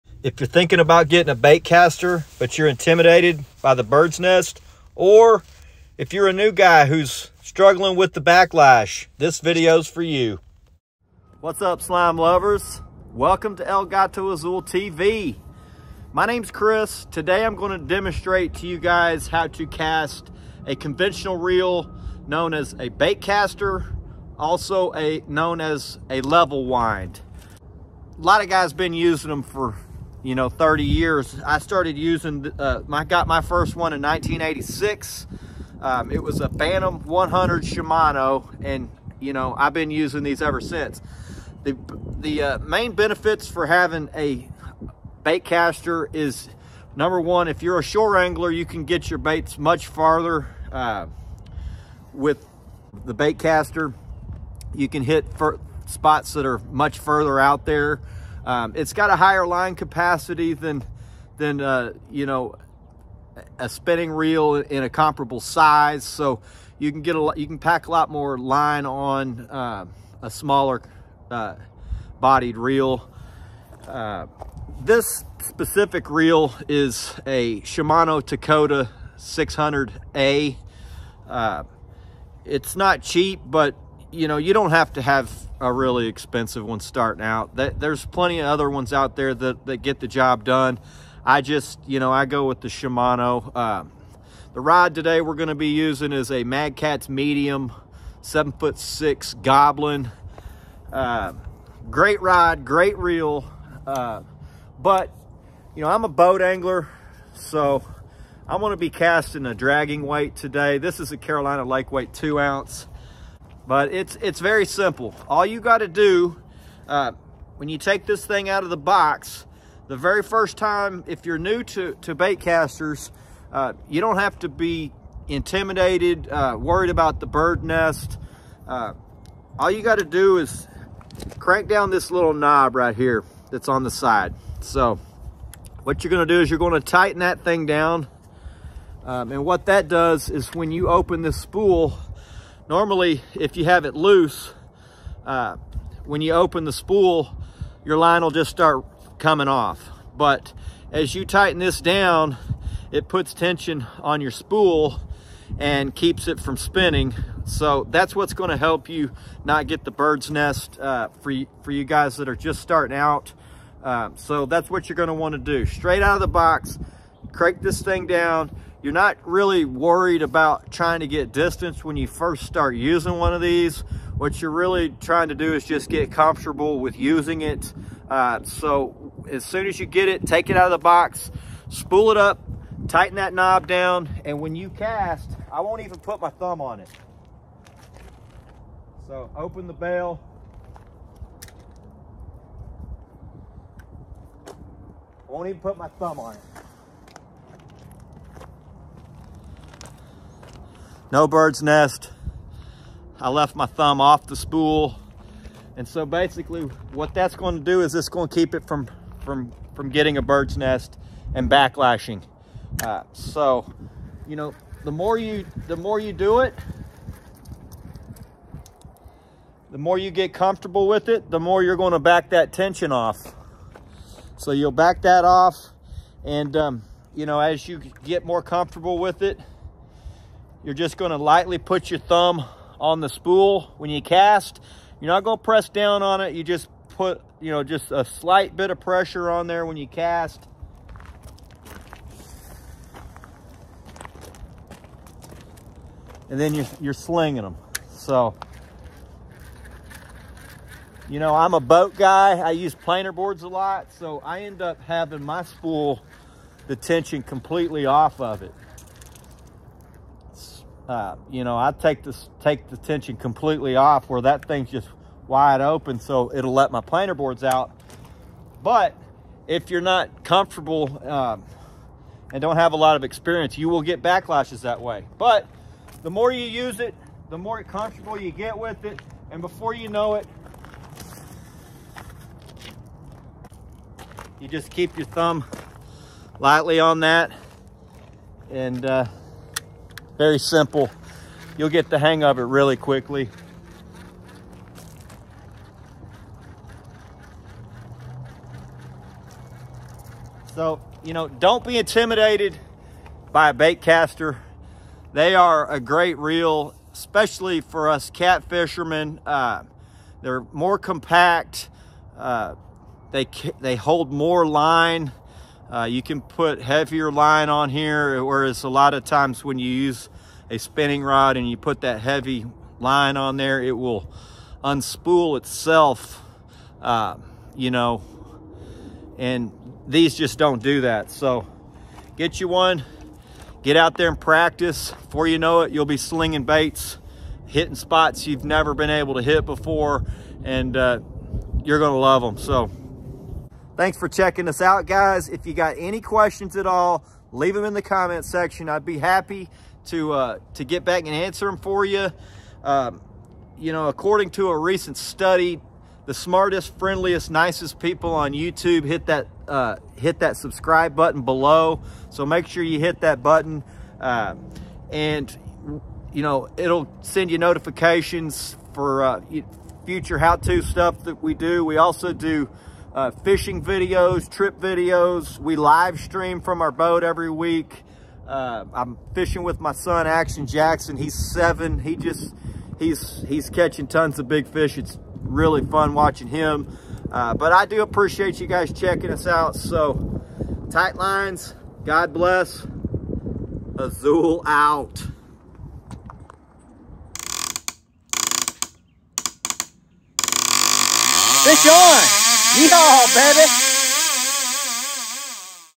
🛑Stop backlash on baitcaster reel. How to cast a level wind and not birds nest conventional reel
baitcasting backlash casting . How to adjust your Baitcasting reel to stop backlash. Easy how-to step by step way to eliminate the birds nest when learning to cast a level wind fishing reel.